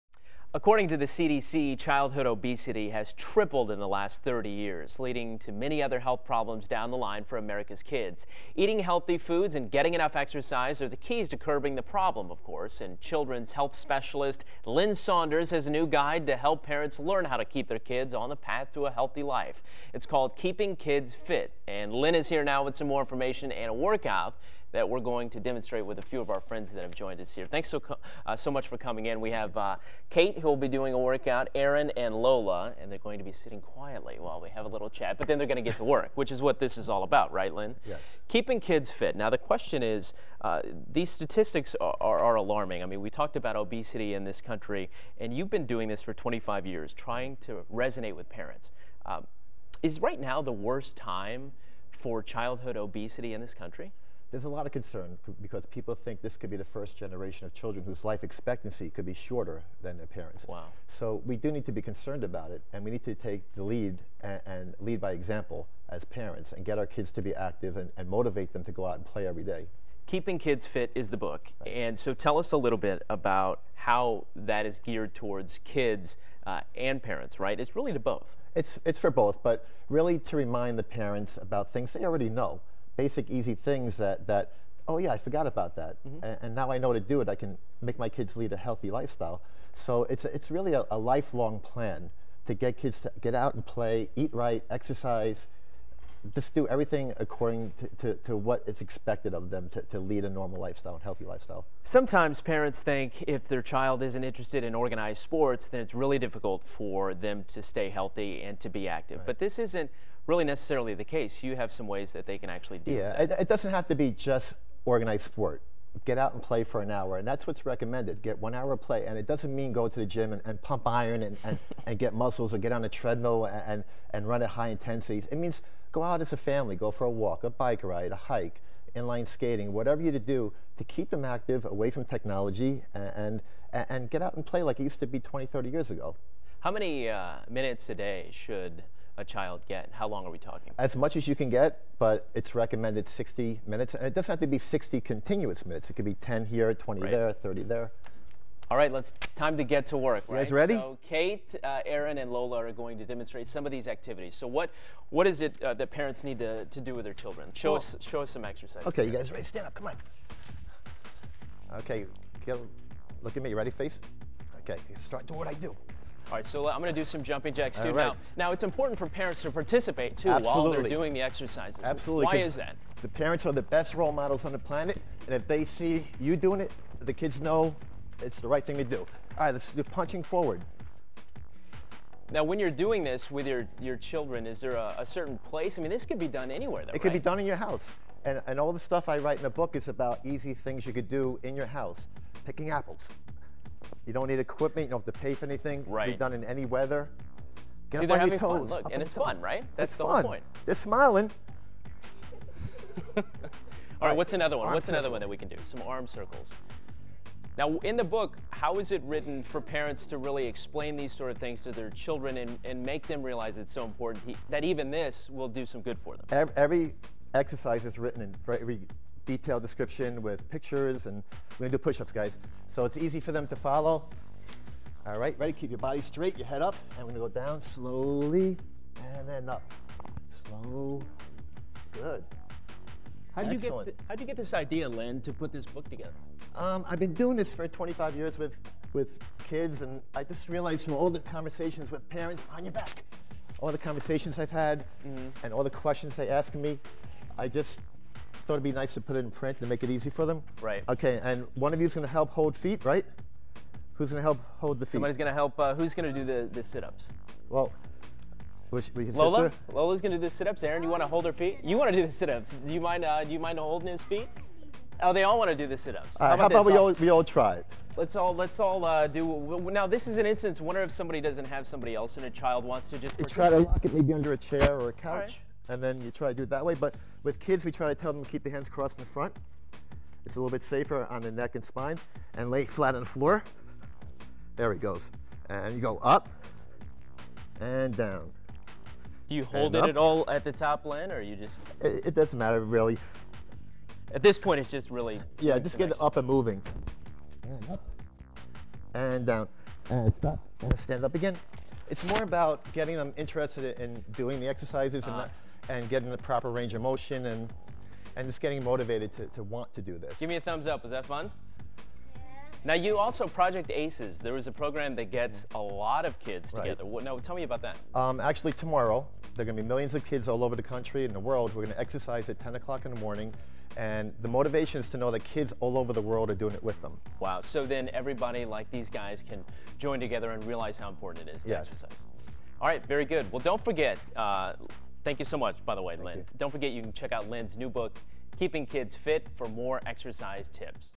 Fitness is For Kids, Too
Tips for teaching your kids healthy habits.